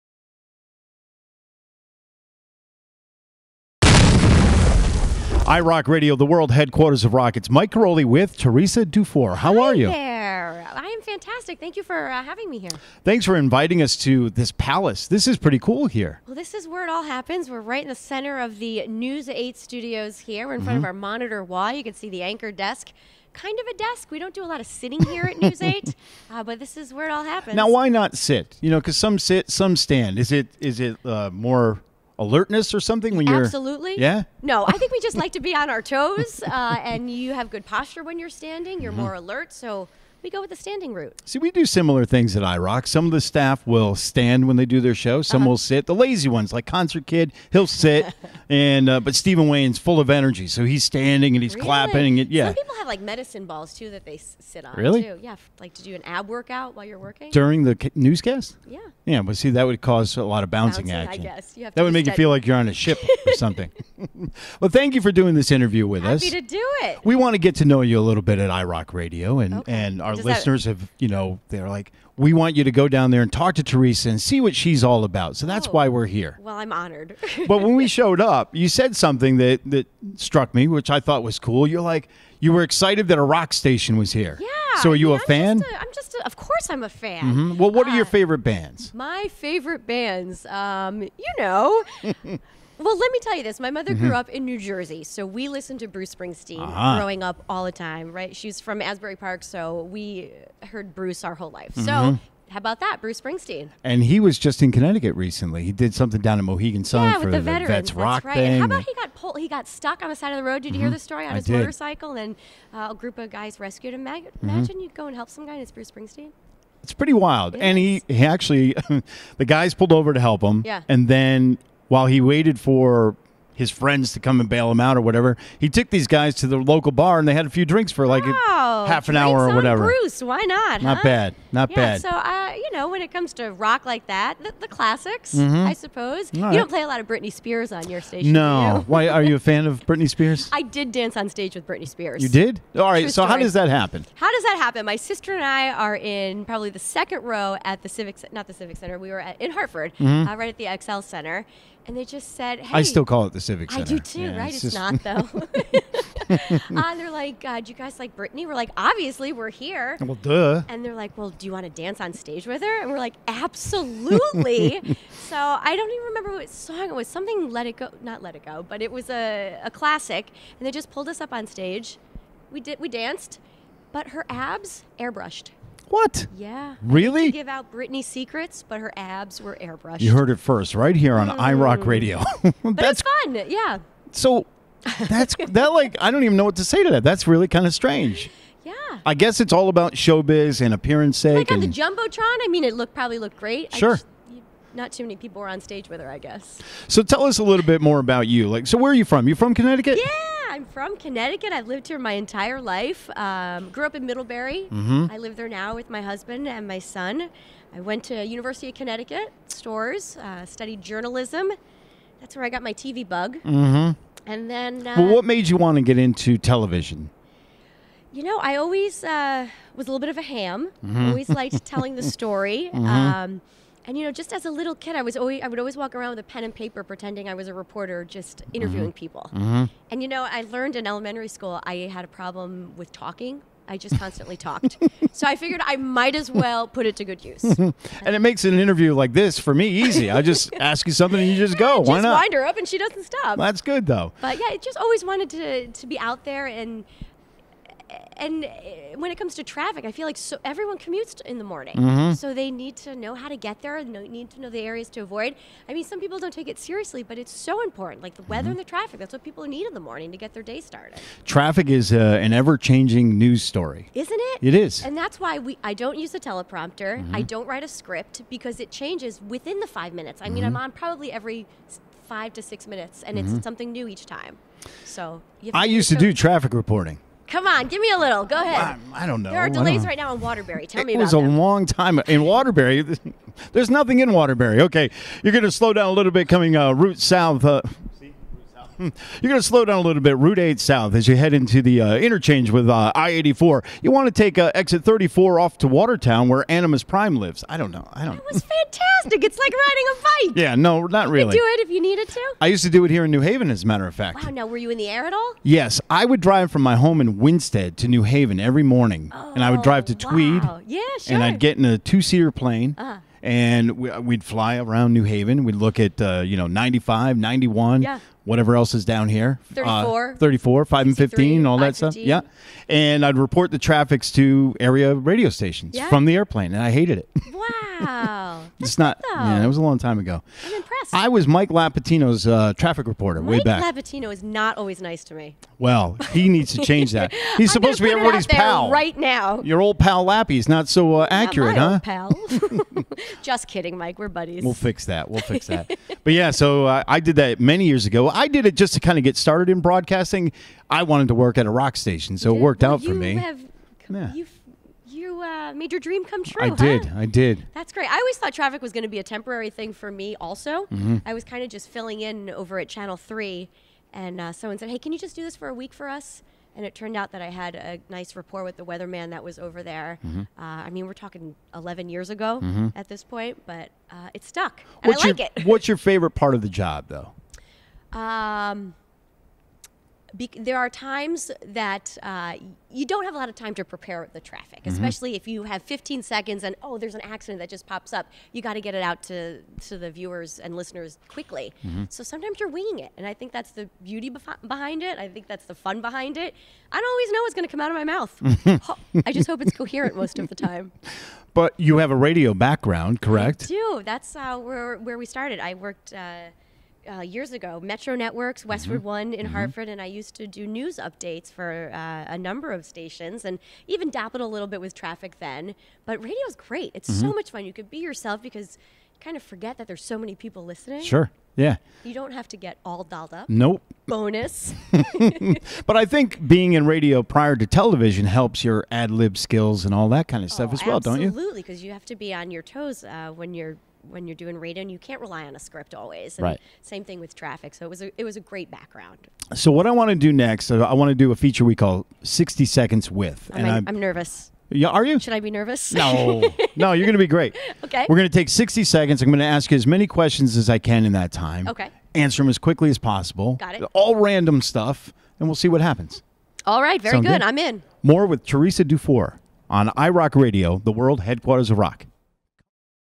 iRockRadio.com- Teresa Dufour - Interview
Karolyi sits down with Teresa Dufour of CT Style and News Channel 8's Good Morning Connecticut